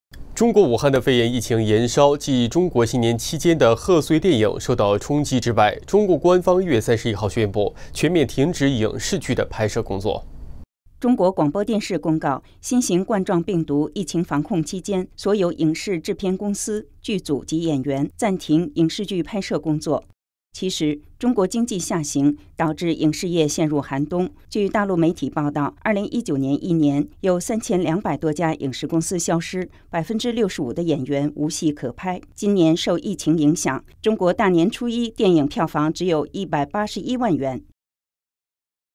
中国影视业雪上加霜！中共宣布停止拍摄【中国禁闻】
【新唐人北京时间2020年02月01日讯】中国武汉肺炎疫情延烧，继中国新年期间的贺岁电影，受到了冲击外，中共官方1月31日宣布，全面停止影视剧的拍摄工作。

中国广播电视公告：「新型冠状病毒疫防控期间，所有影视制片公司、剧组及演员，暂停影视剧拍摄工作。」其实，中国经济下行，导致影视业，陷入寒冬。据大陆媒体报导，2019年一年，有3千2百多家影视公司消失，65%的演员无戏可拍；今年受疫情影响，中国年初一电影票房只有181万元。